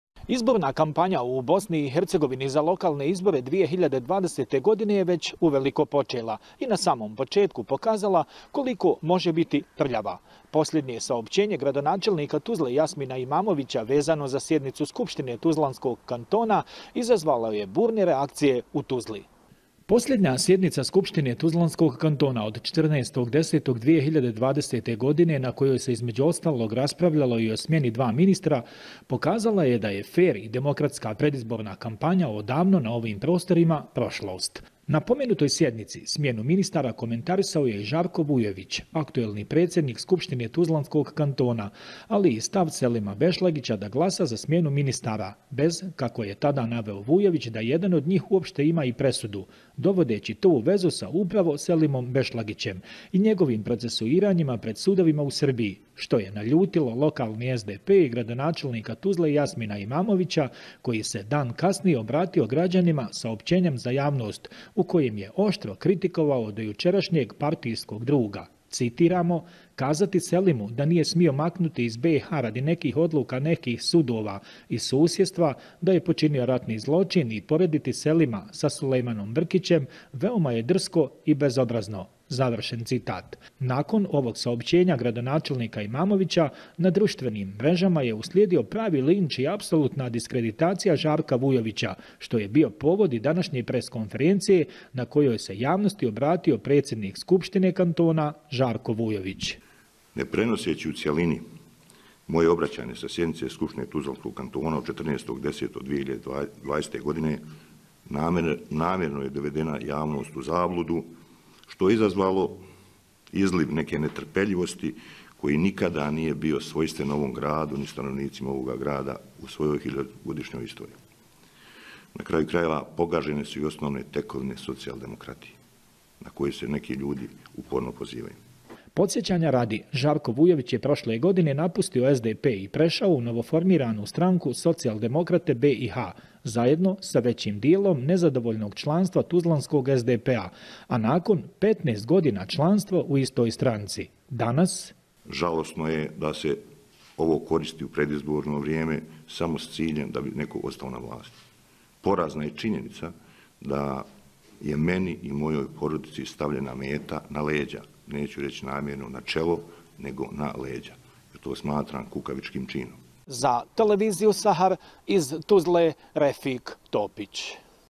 Saopćenja gradonačelnika Tuzle vazano za sjednicu Tuzlanskog kantona izazvala burne reakcije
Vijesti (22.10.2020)